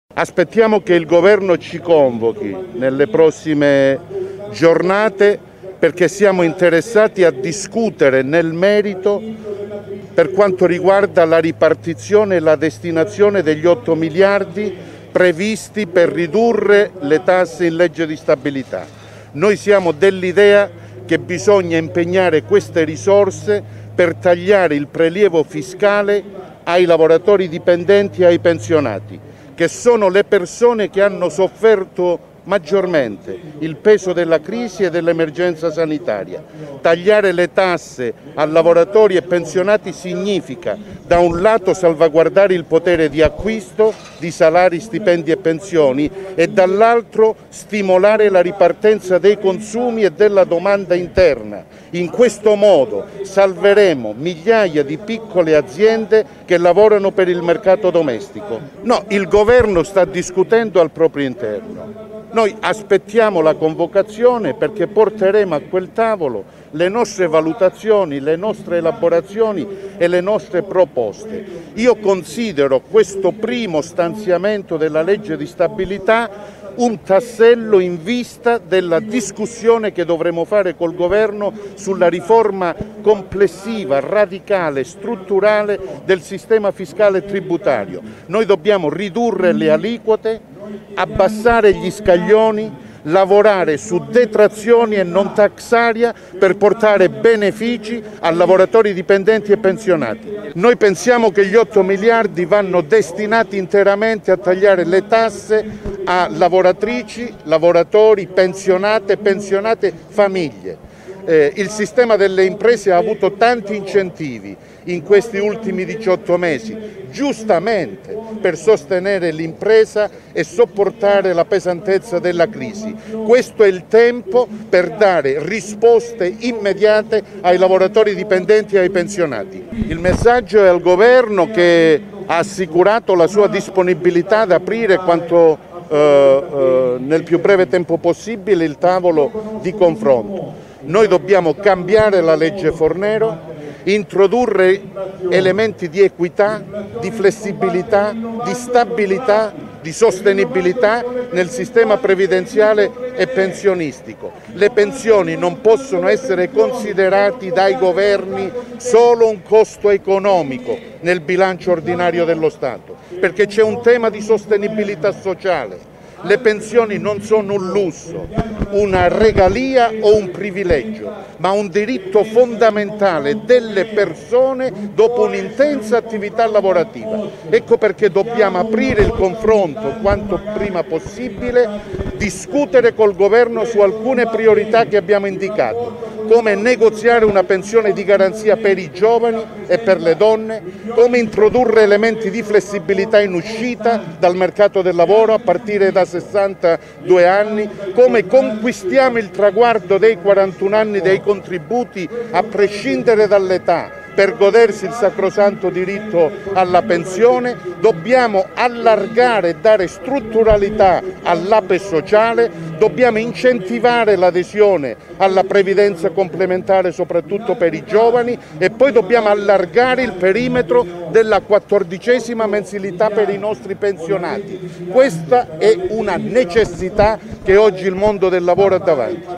Legge Bilancio, Sbarra: "Vogliamo convocazione Governo, partita fisco da risolvere"
Segretario Cisl: "8 miliardi vanno destinati a prelievo fiscale di lavoratori e pensionati"

 (Torino). "Aspettiamo che il Governo ci convochi perché vogliamo discutere nel merito per quanto riguarda la ripartizione degli 8 miliardi previsti per ridurre le tasse in Legge di stabilità - così Luigi Sbarra, segretario generale Cisl, a Torino per la manifestazione dei sindacati contro la Legge di Bilancio - Bisogna impegnare queste risorse per tagliare il prelievo fiscale a lavoratori e pensionati, le persone che hanno patito peggio il Covid. Tagliare le tasse significa salvaguardare il potere di acquisto e stimolare la ripartenza dei consumi, si salverebbero migliaia di piccole aziende. Il Governo discute al proprio interno, noi porteremo a quel tavolo le nostre proposte, io considero questo primo stanziamento della Legge di Stabilità un tassello in vista della discussione da fare sul sistema fiscale". (Sara Iacomussi/alanews)

aspettiamo che il governo ci convochi nelle prossime giornate perché siamo interessati a discutere nel merito per quanto riguarda la ripartizione la destinazione degli 8 miliardi previsti per ridurre le tasse in legge di stabilità Noi siamo dell'idea che bisogna impegnare queste risorse per tagliare il prelievo fiscale ai lavoratori dipendenti ai pensionati che sono le persone che hanno sofferto maggiormente il peso della crisi e dell'emergenza sanitaria tagliare le tasse a lavoratori e pensionati significa da un lato salvaguardare il potere di acquisto di salari stipendi e pensioni e dall'altro la ripartenza dei consumi e della domanda interna in questo modo salveremo migliaia di piccole aziende che lavorano per il mercato domestico No il governo discutendo al proprio interno aspettiamo la convocazione perché porteremo A quel tavolo le nostre valutazioni le nostre elaborazioni e le nostre proposte considero questo primo stanziamento della legge di stabilità un tassello in vista della discussione che dovremmo fare col Governo sulla riforma complessiva del radicale strutturale del sistema fiscale tributario noi dobbiamo ridurre le aliquote abbassare gli scaglioni lavorare su detrazioni e non per portare benefici a lavoratori dipendenti e pensionati noi pensiamo che gli 8 miliardi vanno destinati interamente a tagliare le tasse a lavoratrici lavoratori pensionati pensionati e famiglie il sistema delle imprese ha avuto tanti incentivi in questi ultimi 18 mesi giustamente per sostenere e sopportare la pesantezza della crisi Questo è il tempo per dare risposte immediate ai lavoratori dipendenti ai pensionati il messaggio è al governo che ha assicurato la sua disponibilità ad aprire quanto nel più breve tempo possibile il tavolo di confronto noi dobbiamo cambiare la legge Fornero introdurre elementi di equità di flessibilità di stabilità di sostenibilità nel sistema previdenziale e pensionistico le pensioni non possono essere considerati dai governi solo un costo economico Nel bilancio ordinario dello Stato perché c'è un tema di sostenibilità sociale le pensioni non sono una Regalia o un privilegio ma un diritto fondamentale delle persone dopo un'intensa attività lavorativa Ecco perché dobbiamo aprire il confronto quanto prima possibile discutere col Governo su alcune priorità che abbiamo indicato come negoziare una pensione di garanzia per i giovani e per le donne come introdurre elementi di flessibilità in uscita dal mercato del lavoro a partire da 62 anni come conquistiamo il traguardo dei 41 anni dei contributi a prescindere dall'età per godersi il sacrosanto diritto alla pensione dobbiamo allargare dare strutturalità all'ape sociale dobbiamo incentivare l'adesione alla previdenza complementare soprattutto per i giovani de poi dobbiamo allargare il perimetro della quattordicesima mensilità per i nostri pensionati Questa è una necessità che ho il mondo del lavoro e da me